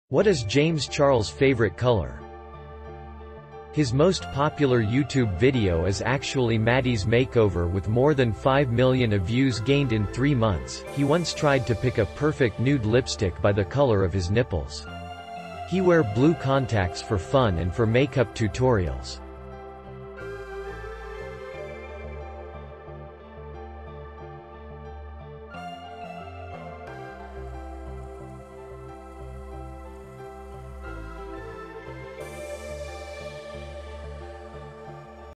What is James Charles favorite color?
His most popular YouTube video is actually Maddie’s makeover with more than 5 million of views gained in three months; He once tried to pick a perfect nude lipstick by the color of his nipples. He wear blue contacts for fun and for makeup tutorials.